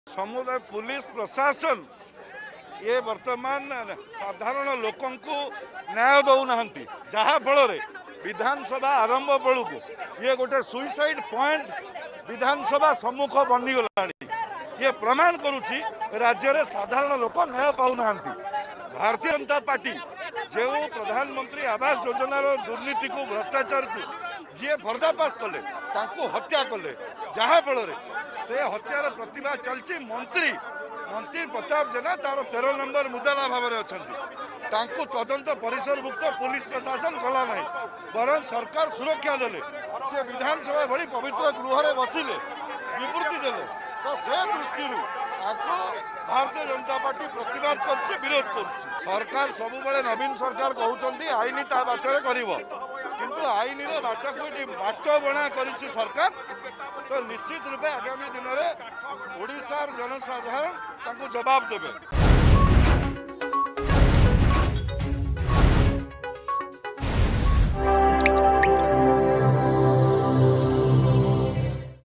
ବିଜେପି ରାଲିରେ ସମୀର ମହାନ୍ତିଙ୍କ ପ୍ରତିକ୍ରିୟା....
ଆଜି ବିଧାନସଭା ଅଭିମୁଖେ ବିଜେପି ରାଲିରେ ସମୀର ମହାନ୍ତିଙ୍କ ପ୍ରତିକ୍ରିୟା.